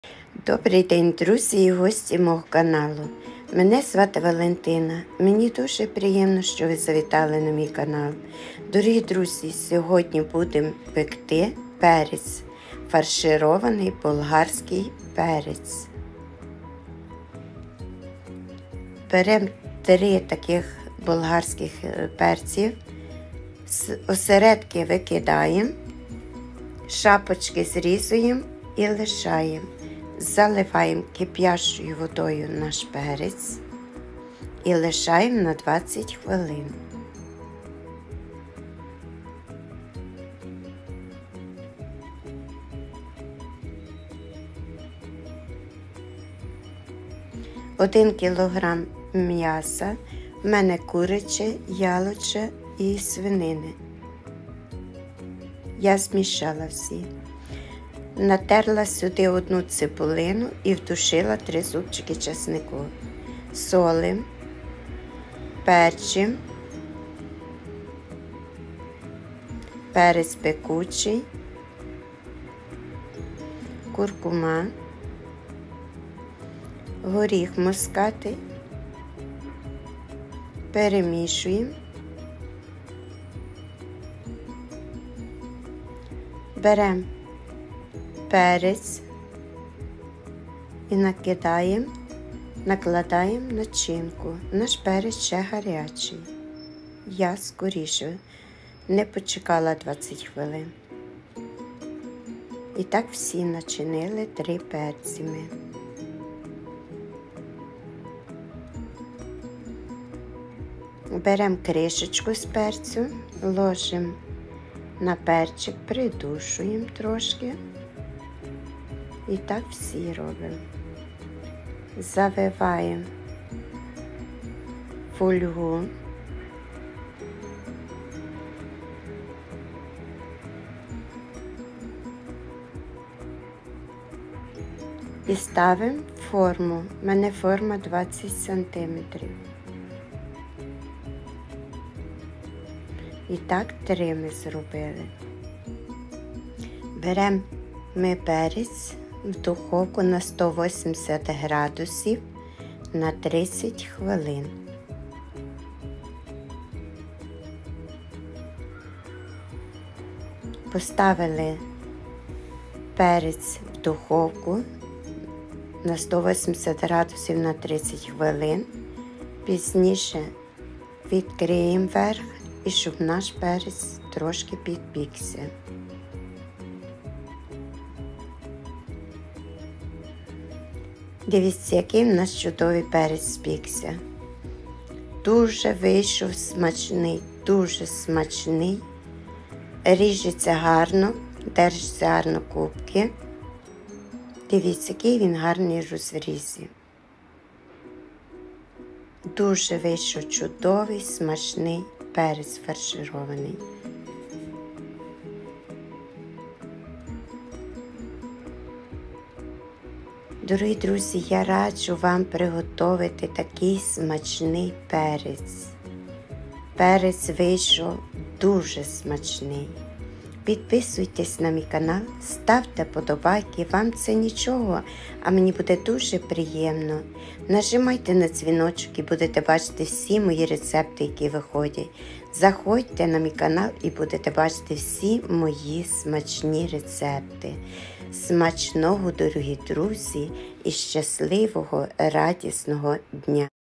Чудовий фарширований перець: неймовірний аромат!
EASY COOK TV - кулінарний влог Валентини Мінковської
★ Прості відеорецепти оригінальних страв
★ Традиційні страви української кухні - пальчики оближеш!

Інгредієнти:

✅ 3 болгарські перці
✅ 1 кг фарш
✅ 1 цибулина
✅ 3 зуб. часника
✅ сіль
✅ перець
✅ перець пекучий
✅ куркума
✅ горіх мускатний

180 градусів - 30 хв.